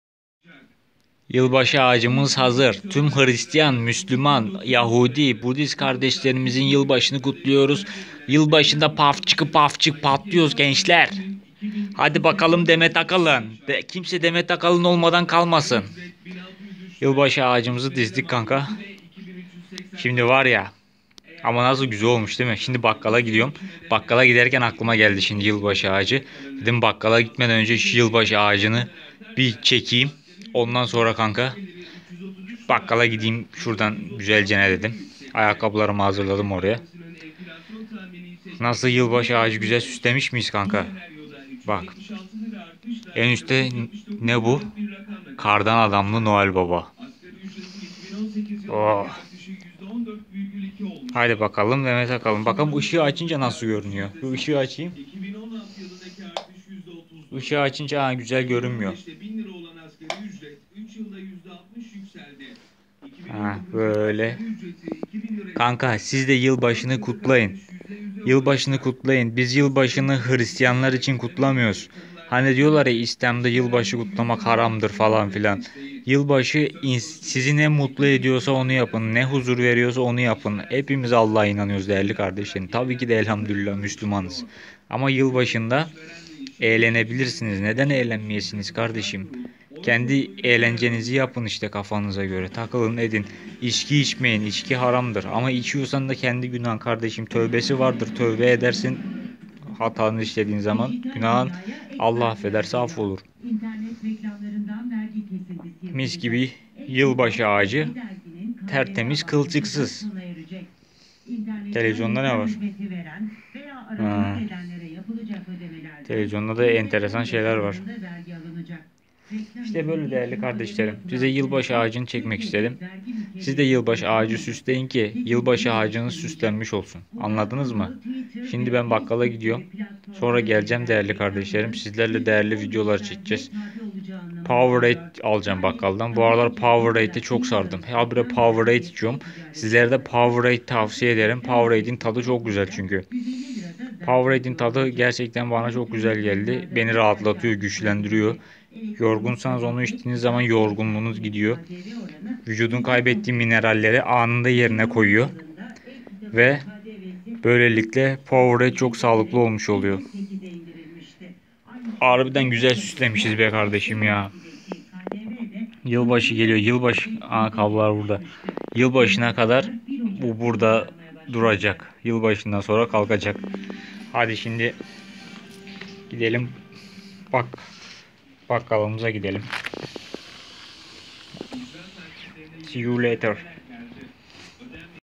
Yılbaşı geliyor Ağacımızı hazırladık! Muhteşem oldu!
Yılbaşı ağacımızı hazırladık! Sizce nasıl?